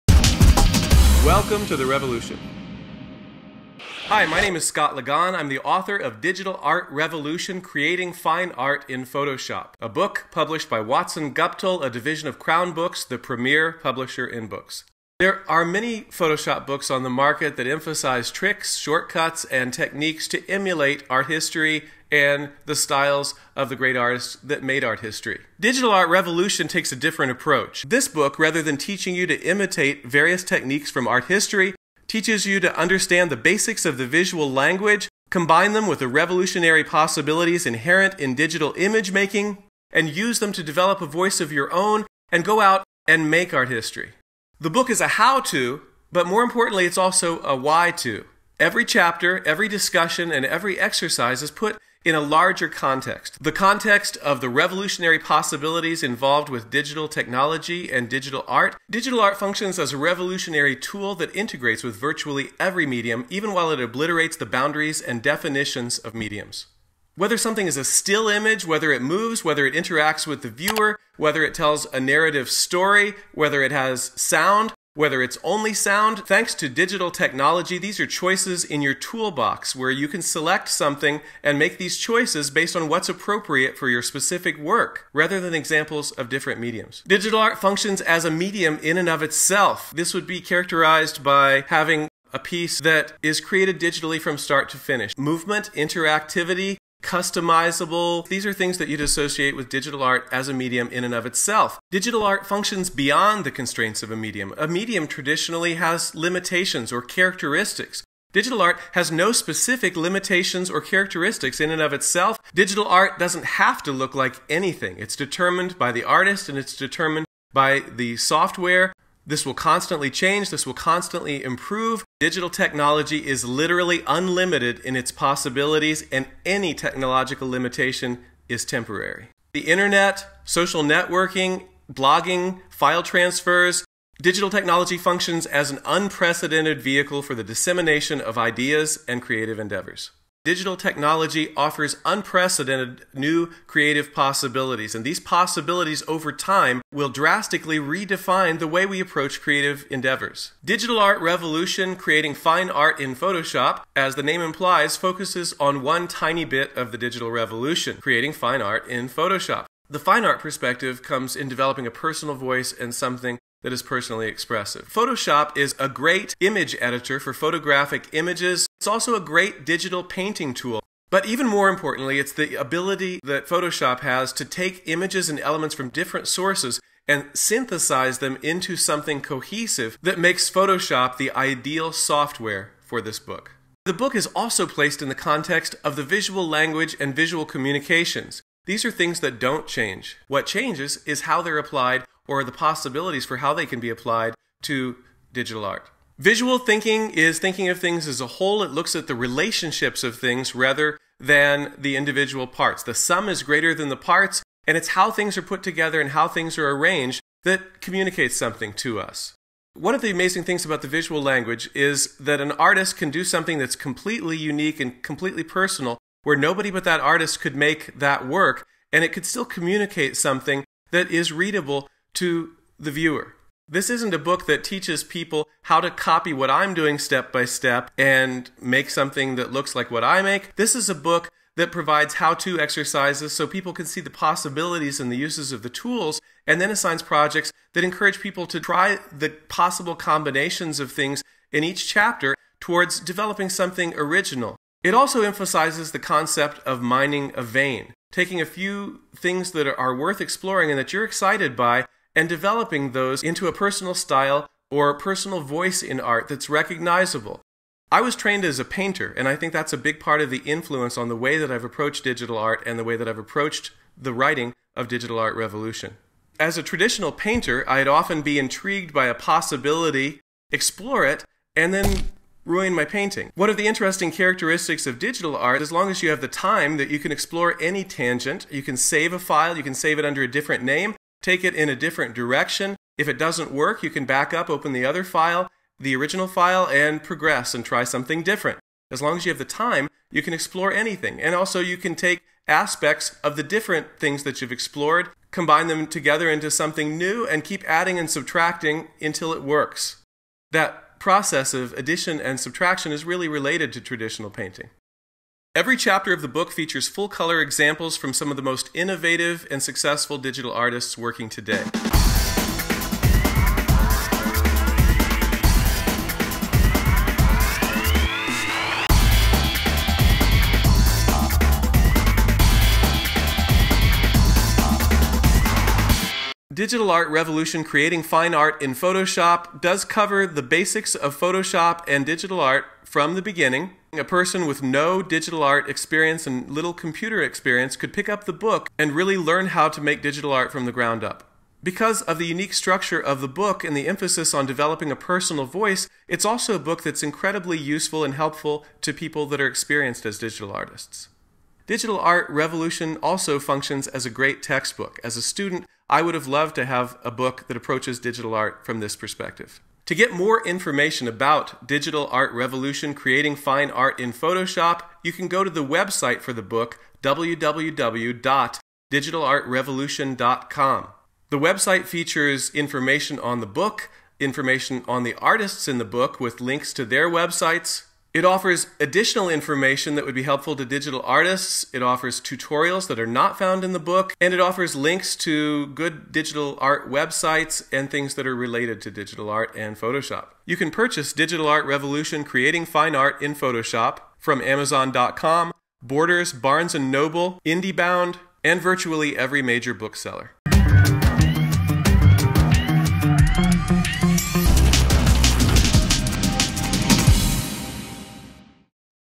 How Digital Art Will Change the World! - "Digital Art Revolution: Creating Fine Art in Photoshop"
A video for the book "Digital Art Revolution: Creating Fine Art with Photoshop" by Scott Ligon. The video discusses the world changing characteristics of digital art. "Digital Art Revolution" features some of the most innovative and successful digital artists working today.

Software used to create this video:
Adobe Photoshop
Adobe After Effects
Adobe Illustrator